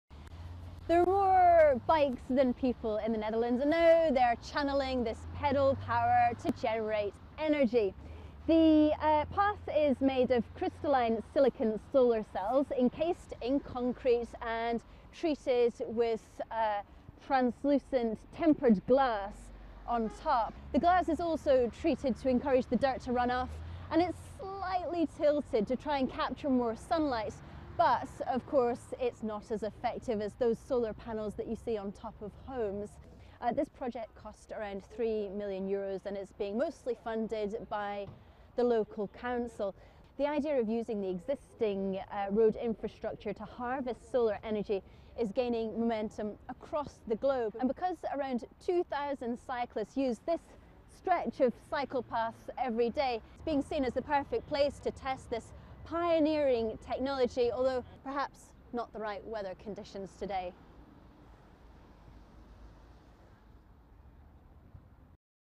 Solar cycle path opens in Netherlands
The world's first public solar cycle lane has opened in the Netherlands as part of a pilot scheme.

It is designed to capture the energy from the sun and transform it into electricity.

During the pilot, the generated energy will be supplied to the electricity grid, just like most solar panels on rooftops.

It is expected that the pilot cycle path of approximately 100m (330 ft) will generate as much electricity as is used by two to three average households annually.

Anna Holligan visited the cycle path north of Amsterdam for a closer look.